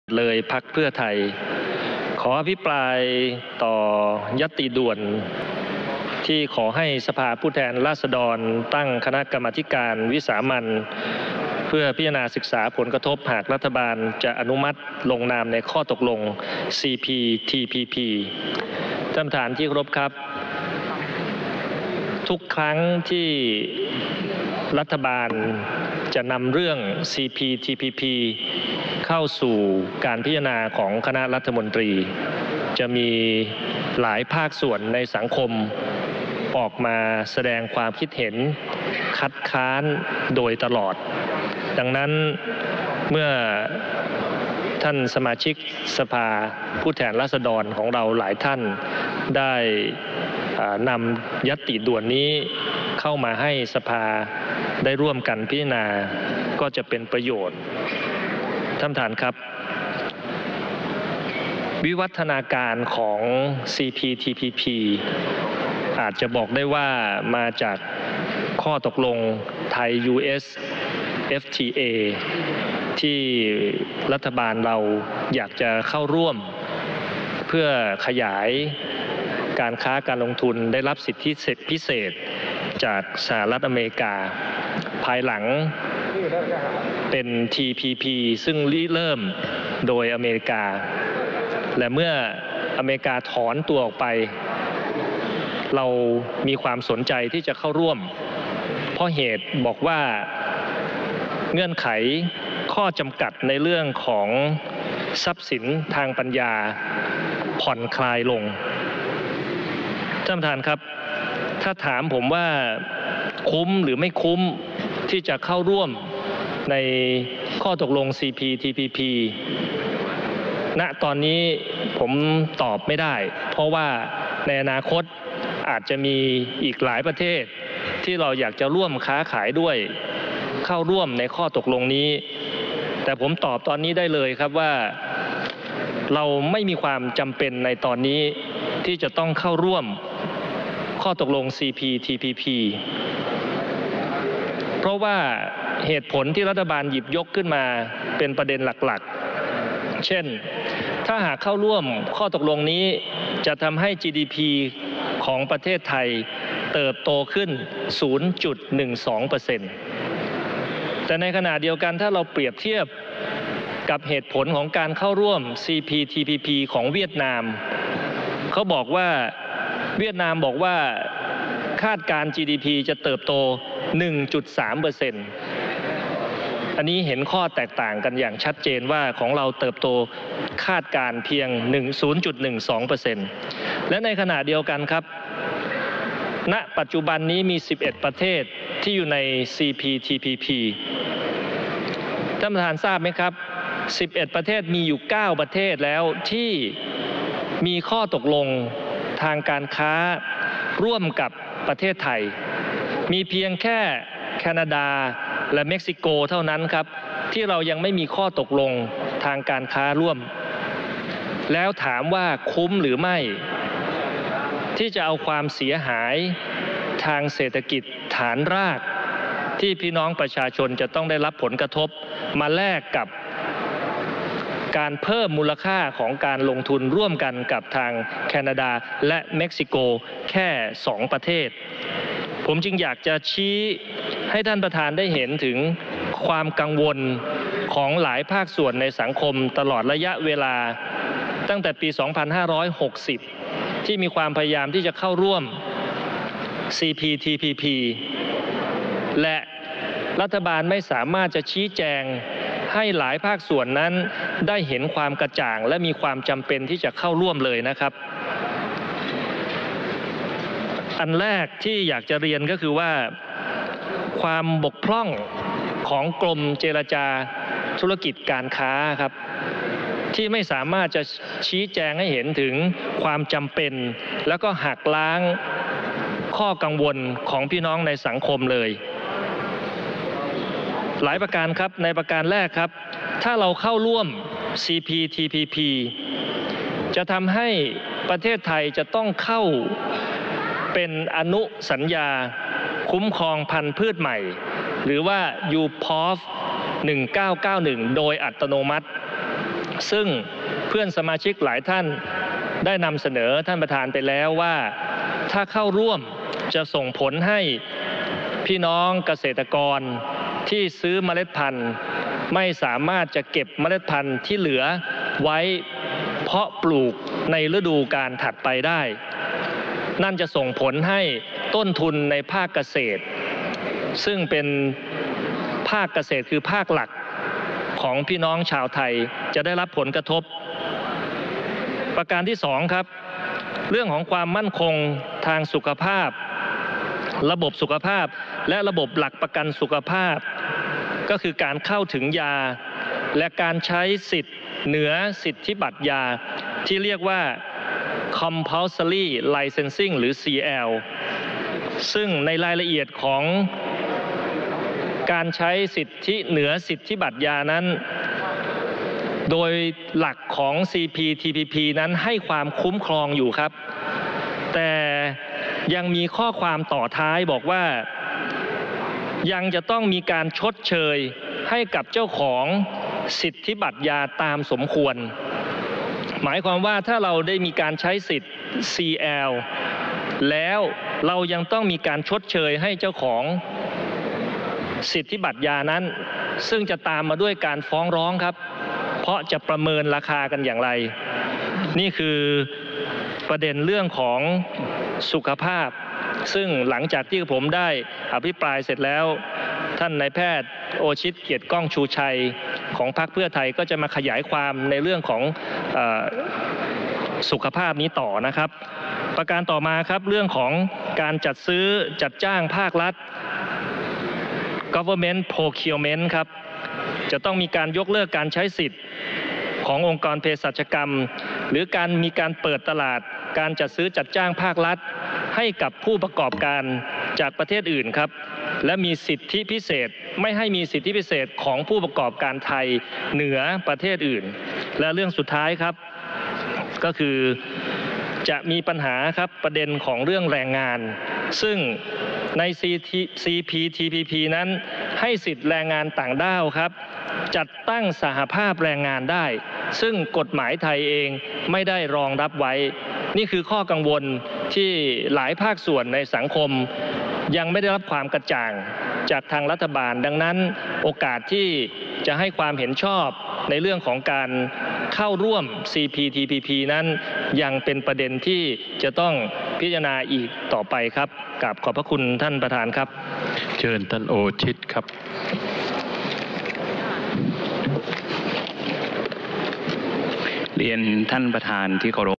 นายเลิศศักดิ์ พัฒนชัยกุล อภิปรายญัตติตั้งคณะกรรมาธิการวิสามัญพิจารณาศึกษาผลกระทบของ CPTPP 10/6/63
นายเลิศศักดิ์ พัฒนชัยกุล ส.ส.เลย พรรคเพื่อไทย อภิปรายญัตติตั้งคณะกรรมาธิการวิสามัญพิจารณาศึกษาผลกระทบ หากรัฐบาลจะอนุมัติลงนามในข้อตกลงที่ครอบคลุมและก้าวหน้าสำหรับหุ้นส่วนทางเศรษฐกิจภาคพื้นแปซิฟิก หรือ CPTPP

การประชุมสมาชิกสภาผู้แทนราษฏร ครั้งที่ 3 เป็นพิเศษ

วันพุธที่ 10 มิถุนายน 2563